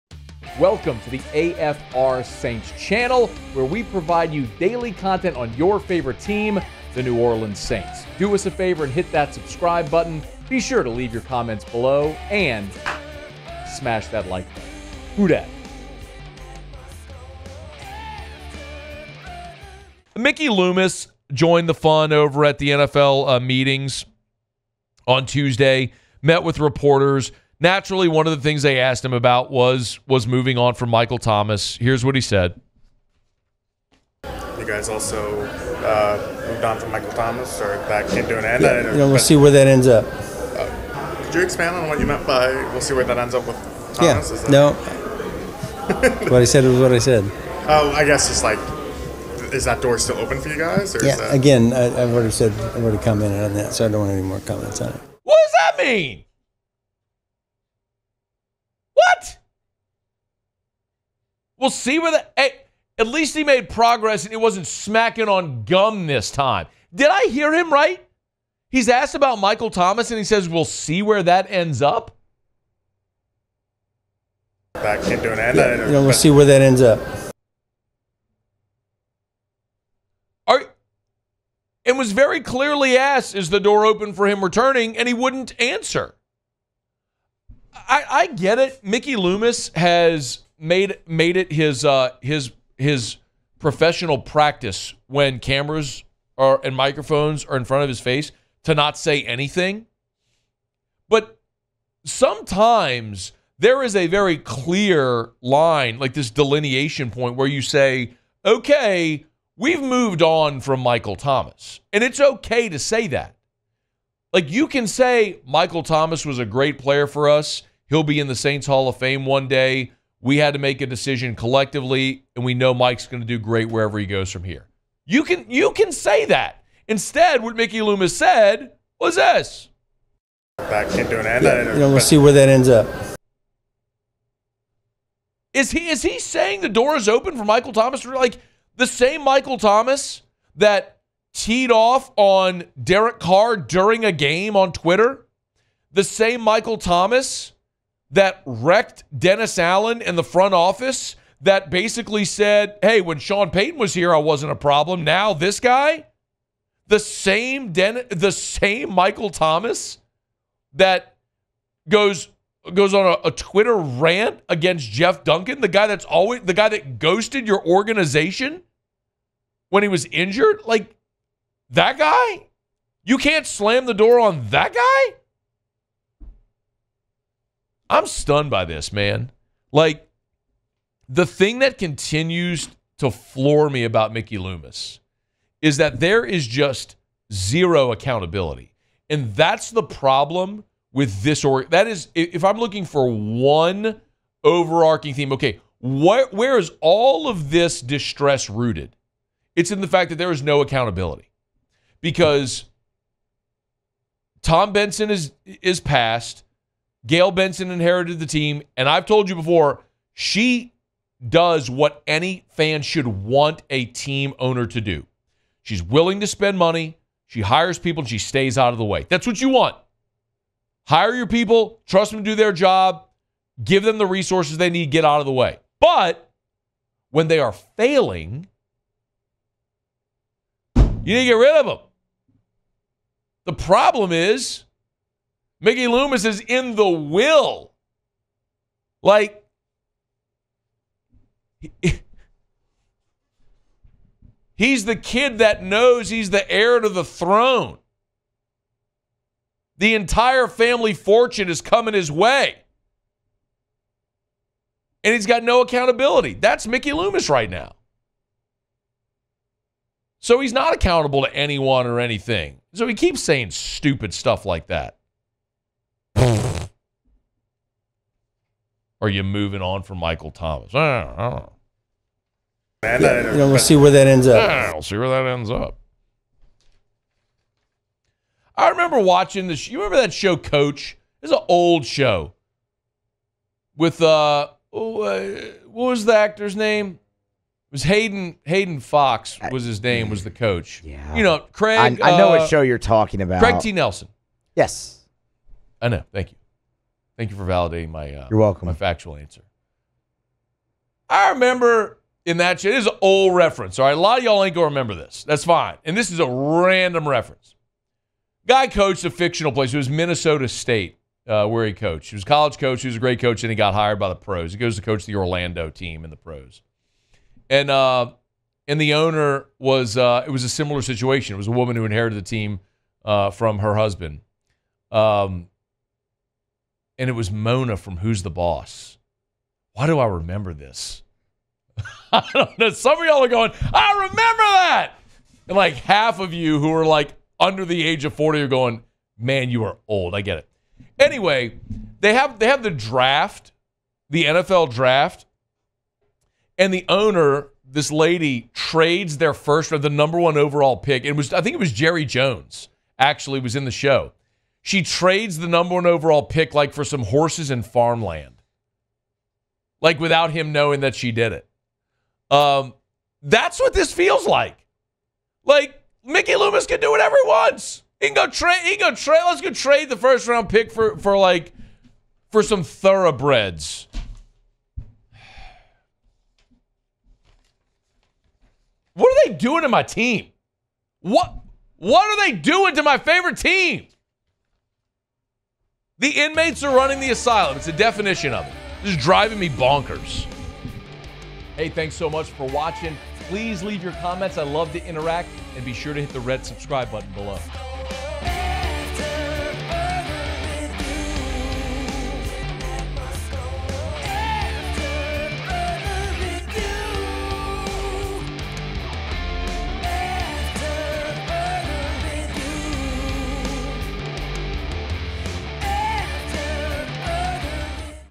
Has Saints GM Mickey Loomis Lost ALL CREDIBILITY?!?!? | Time To Move On In New Orleans!
Saints GM Mickey Loomis spoke to reporters at the NFL owners meetings and discussed the Saints offseason. He discussed the Saints salary cap situation and the release of WR Michael Thomas. Matt Moscona reacts to his answers and explains why Mickey Loomis has no fear of consequences.

Photo Credit: Michael C. Hebert/New Orleans Saints